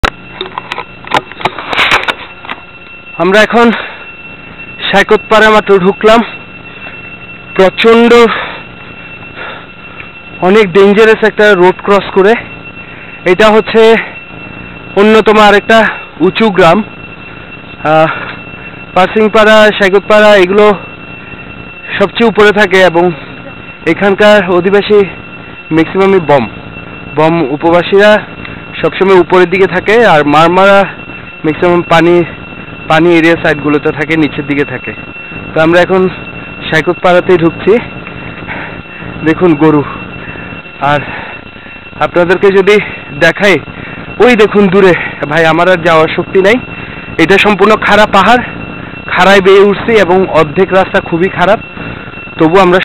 Bandarban Travel
tried to show bandarban tour, it was seriously rough & tough, oh also cross all limits.